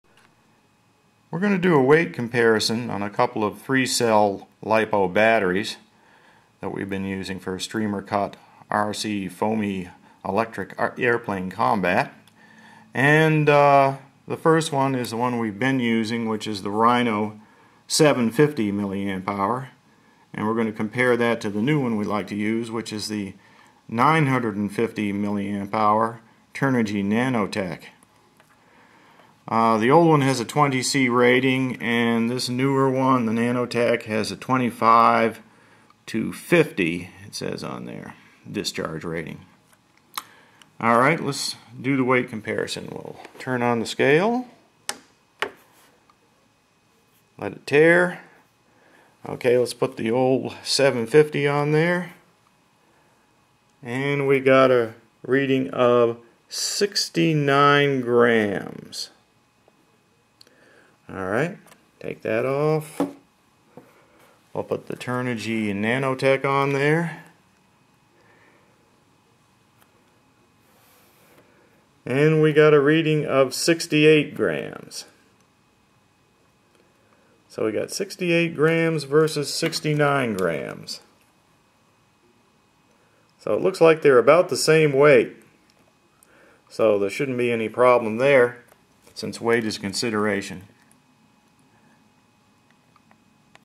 Comparing 3 Cell Nanotech 950 LiPo with Rhino 750 battery Weights
We knew the Turnigy Nanotech 950 mAh battery was more powerful than the Rhino 750 mAh battery but what about the weight?
68 to 69 grams same as the Rhino 750.